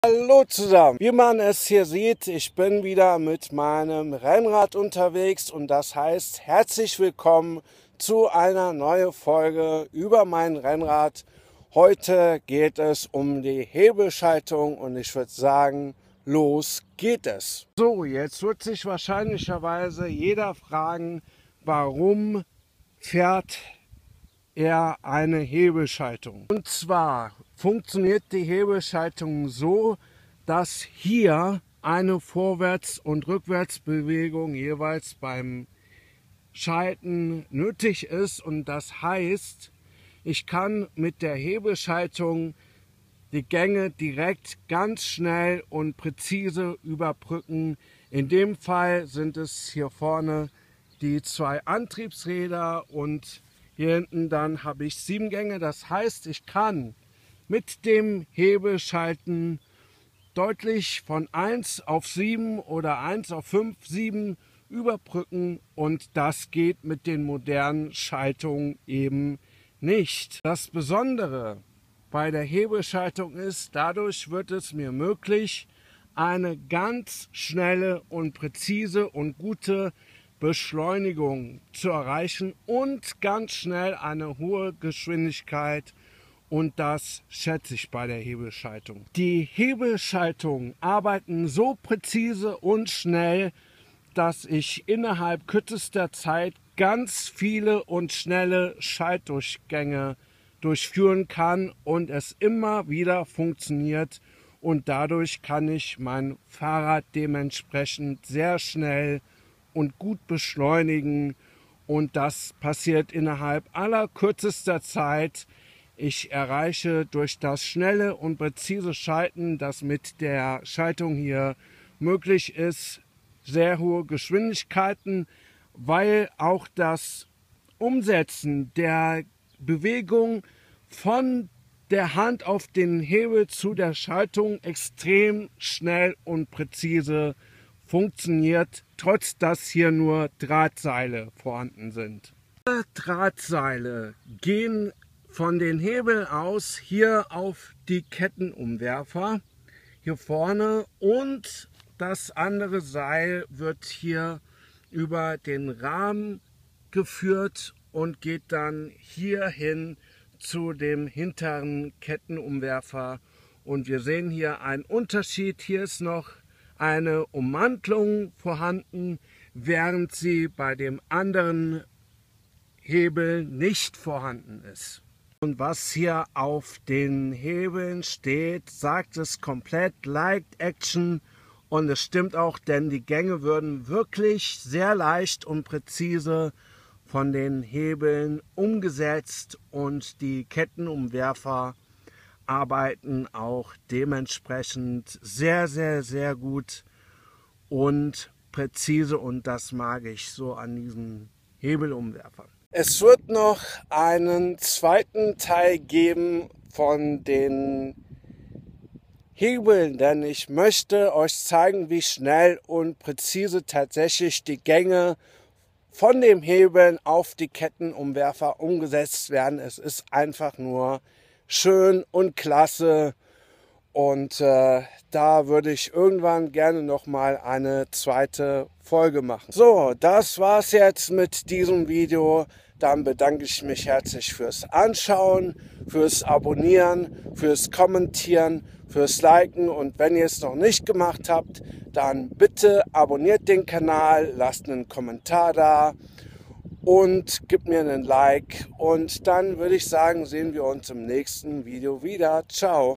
Das Rennrad Und Seine Technik- Hebel/Rahmenschaltung
Was ist eine Hebelschaltung bzw Rahmenschaltung? Wie funktioniert sie? Warum fahre ich ein altes Rennrad mit Hebelschaltung? In diesem Video gibts antworten auf diese Fragen